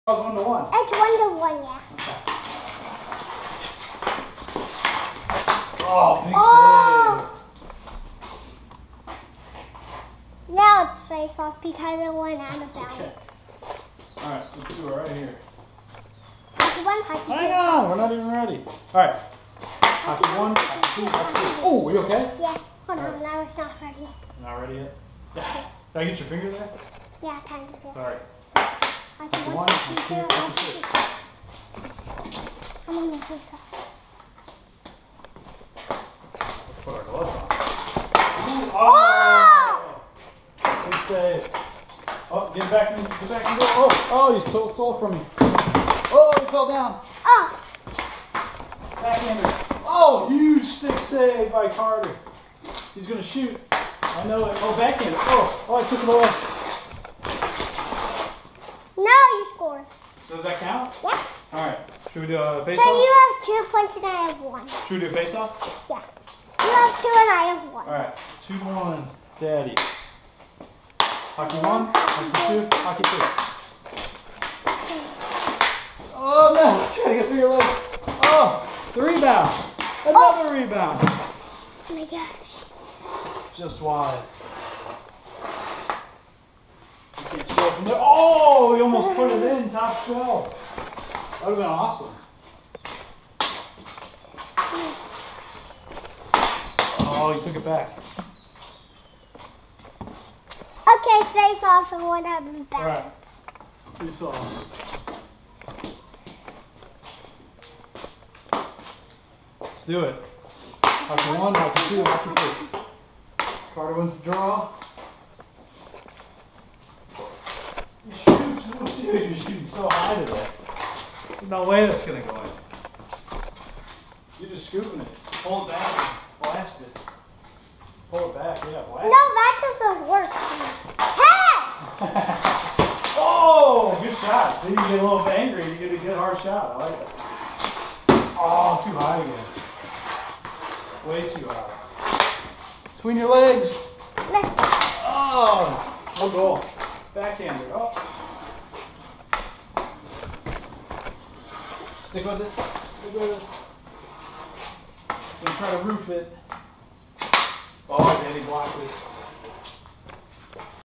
Carter POV Garage Hockey!
Helmet Cam on Carter for garage hockey against Dan, with help from the shooter tutor